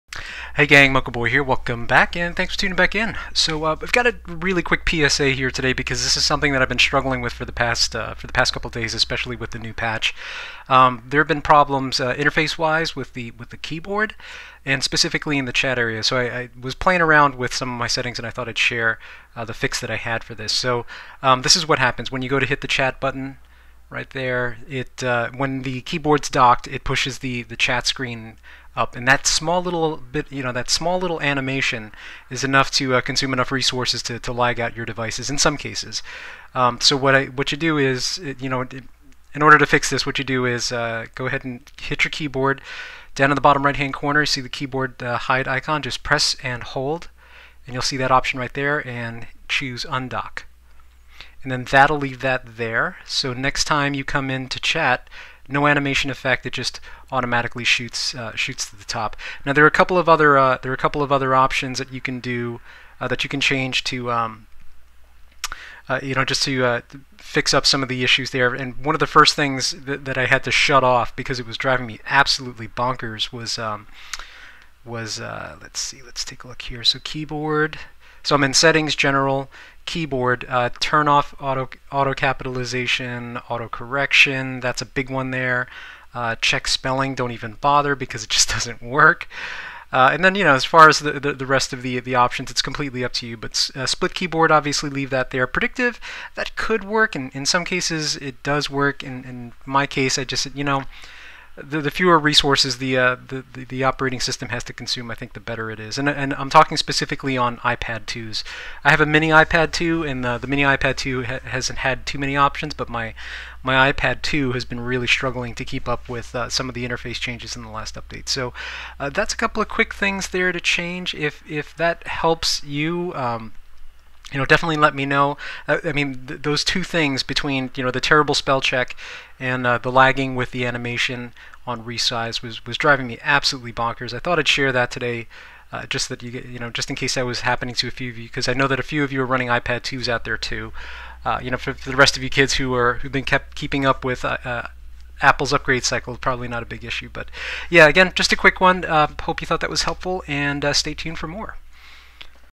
PSA - Minimizing Keyboard Lag Issues Post Patch
Undock your keyboard
Settings - General - Keyboard - shut off auto correct and spell check
collect 3 stars